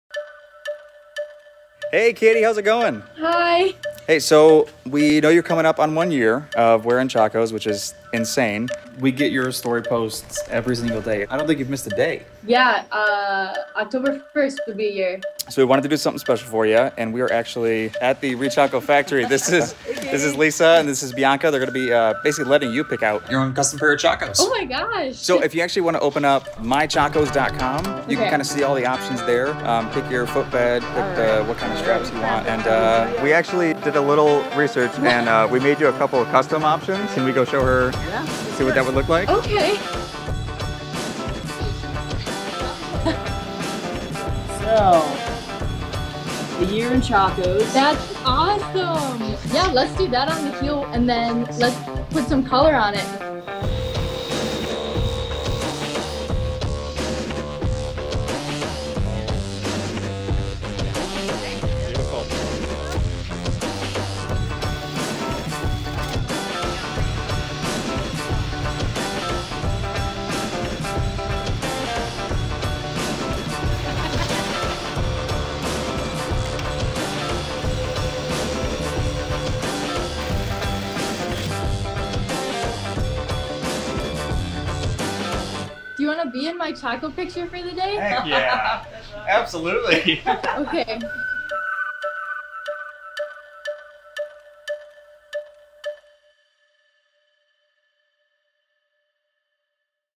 A Year in Chacos
The ChacoNation’s dedication is nuts (in the very best way); that’s nothing new. You take your sandals and boots to higher heights and deeper trails than any other footwear you own. Every mark, every worn sole, every bump in the road tells a story with your shoes beneath you. And our new friend Katie B is no different.

On October 1st, 2019 Katie decided she was going to spend a year in Chacos, rain or shine. She tagged us in her daily Instagram stories, taking us through her adventurous summer and socko filled winter. Our social media team took notice, and wanted to surprise her with a little something to commemorate the moment, and thank her for the incredible dedication and love for the brand. It’s people just like Katie B. (and you) that keep our team going day in and day out, all around the world.

Keep being amazing and sharing all of your incredible adventures with us, ChacoNation. You never know what we might notice next 👀